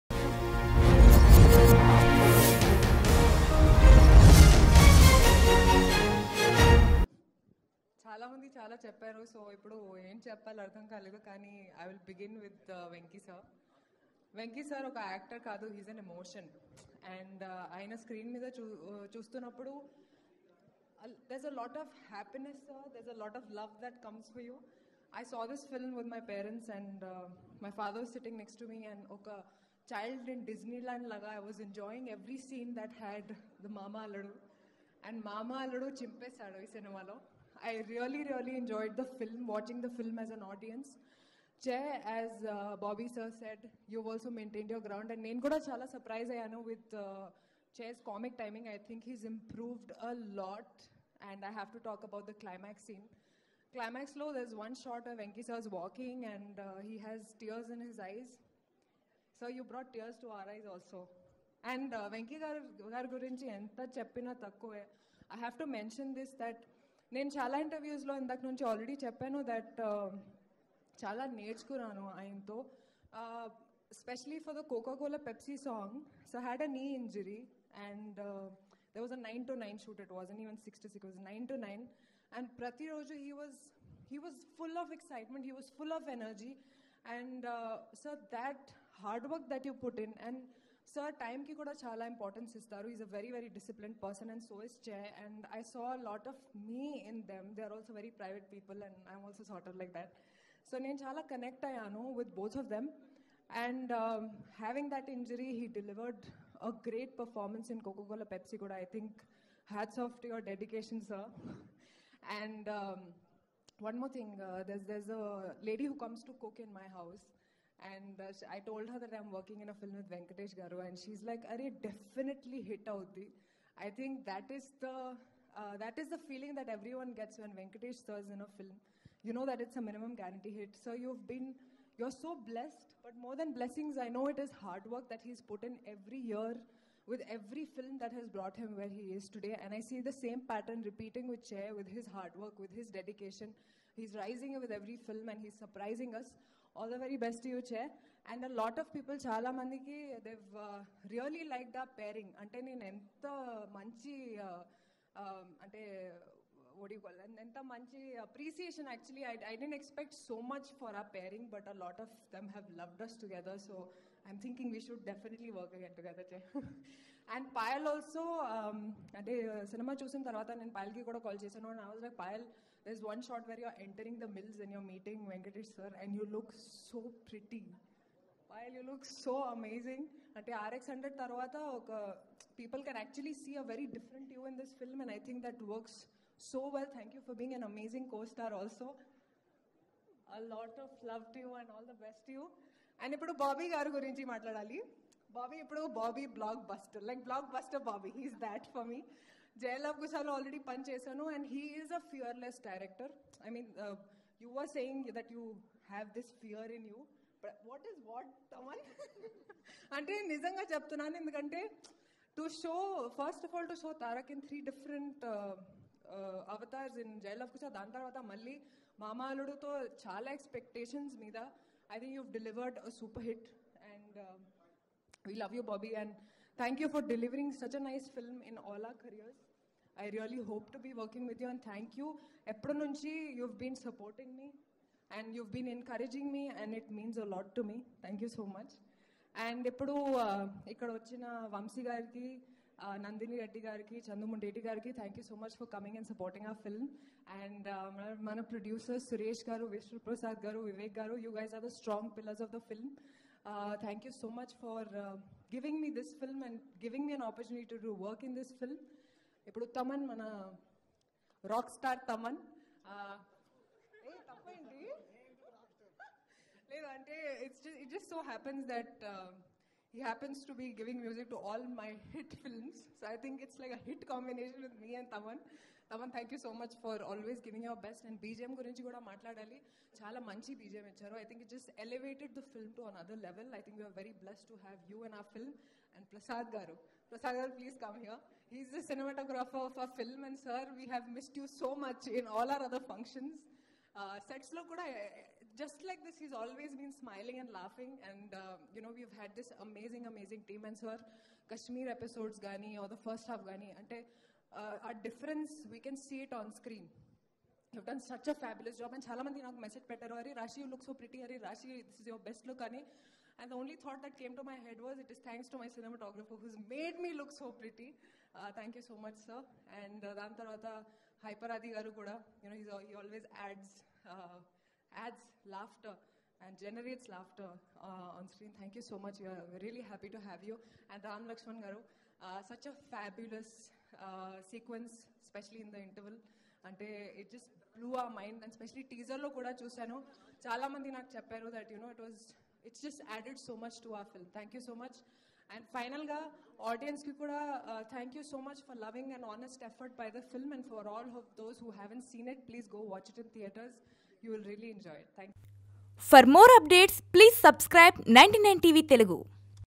Raashi Khanna Speech In Venky Mama Success Celebration | Venkatesh | 99 TV Telugu
actress raashi khanna speech in venky mama success celebration.